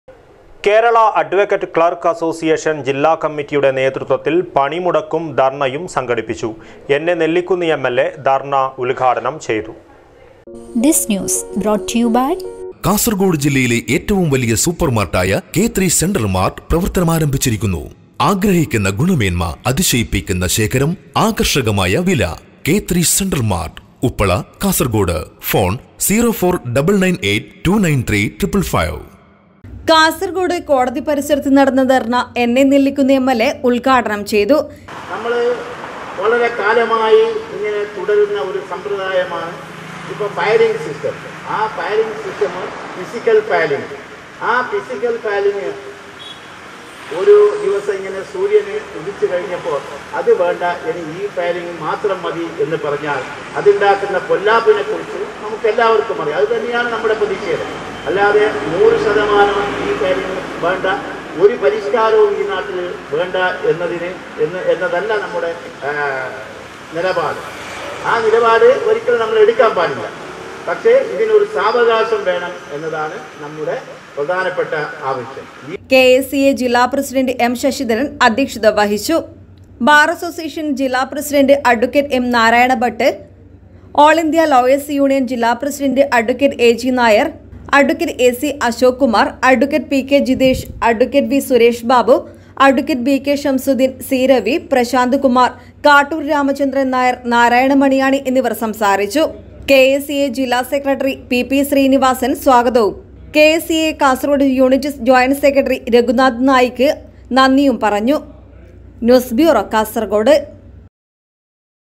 കേരള അഡ്വക്കേറ്റ് ക്ലാര്‍ക്ക് അസോസിയേഷന്‍ പണിമുടക്കും ധര്‍ണയും സംഘടിപ്പിച്ചു
കേരള അഡ്വക്കേറ്റ് ക്ലാര്‍ക്ക് അസോസിയേഷന്‍ ജില്ലാ കമ്മിറ്റിയുടെ നേതൃത്വത്തില്‍ പണിമുടക്കും ധര്‍ണയും സംഘടിപ്പിച്ചു. എന്‍ എ നെല്ലിക്കുന്ന് എം എല്‍ എ ധര്‍ണ്ണ ഉദ്ഘാടനം ചെയ്തു.